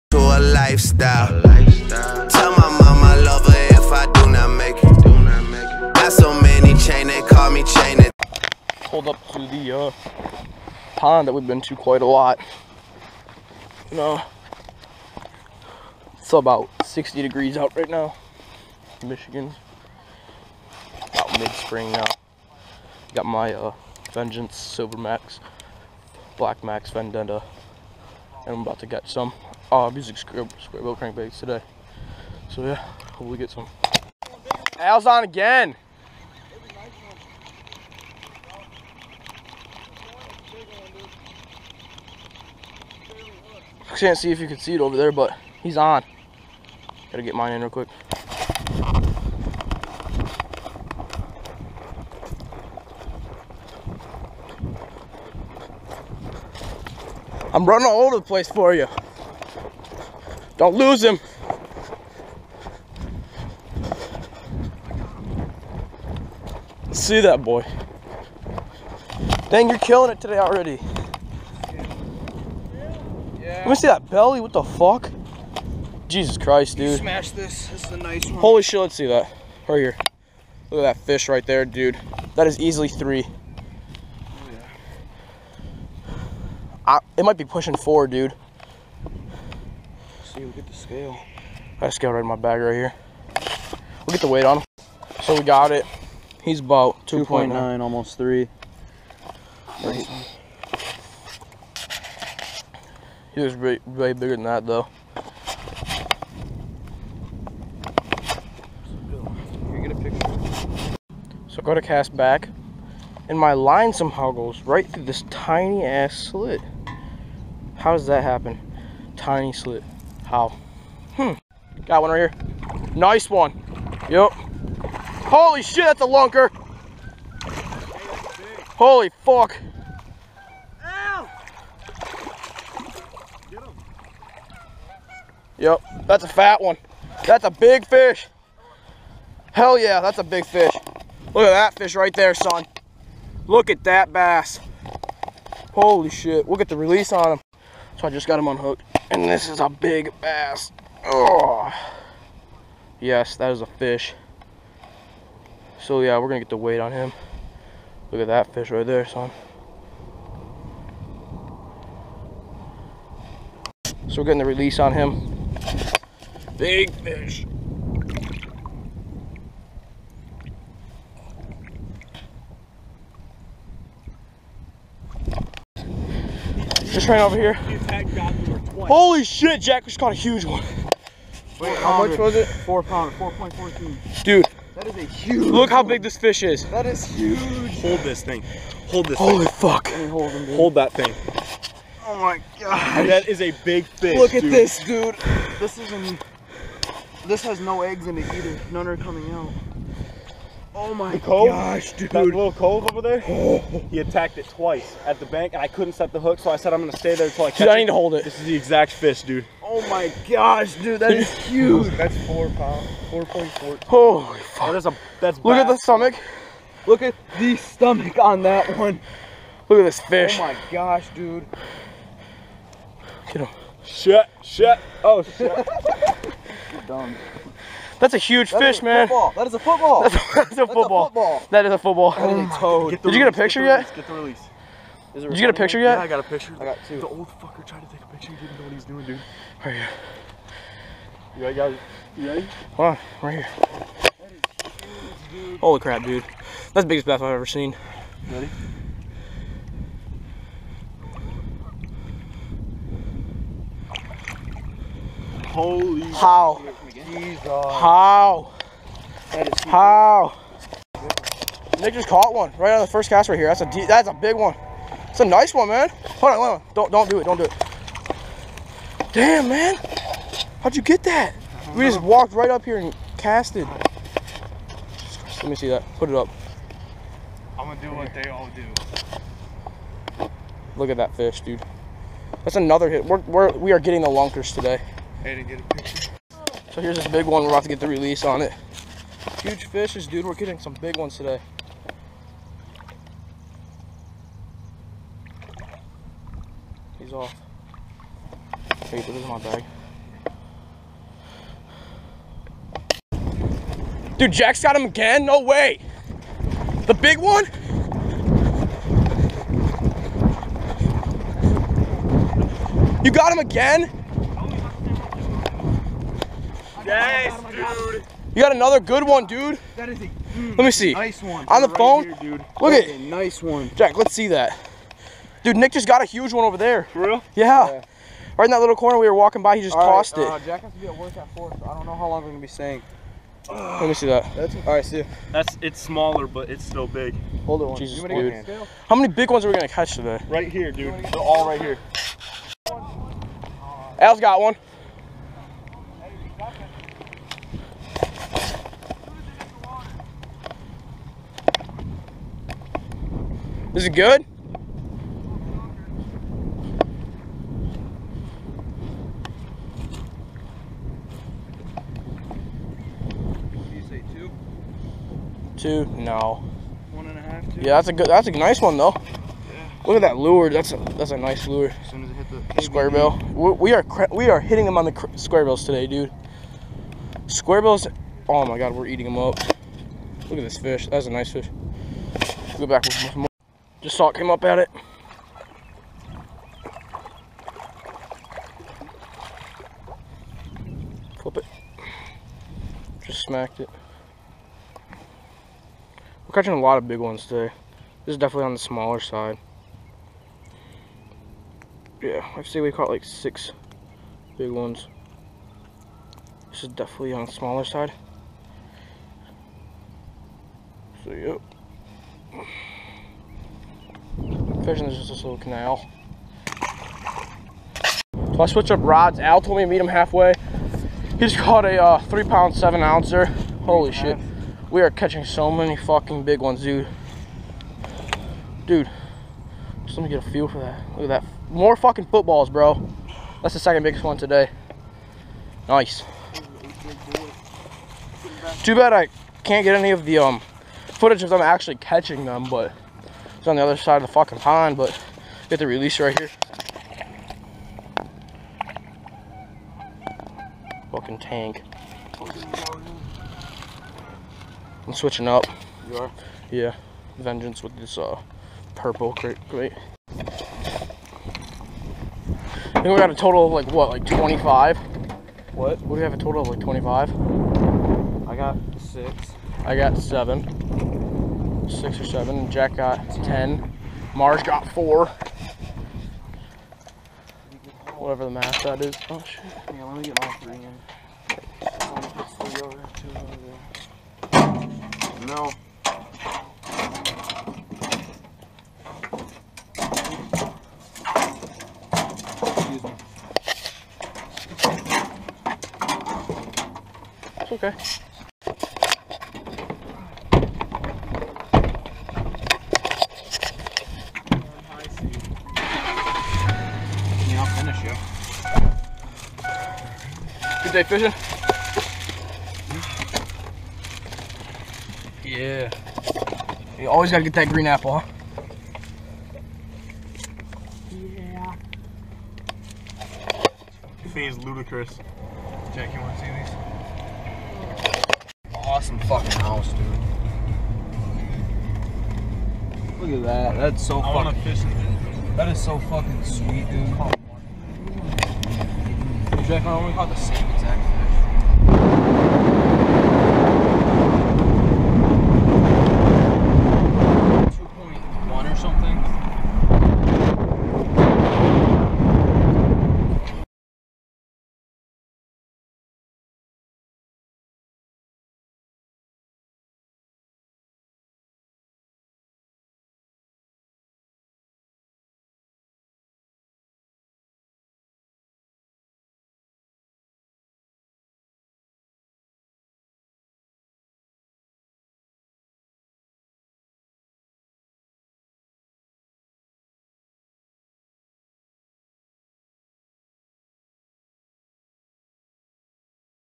CATCHING HUGE LARGEMOUTH BASS ALL DAY (NO DINKS)
Fishing a familiar pond with squarebill crankbaits.  Huge 5 pounder caught.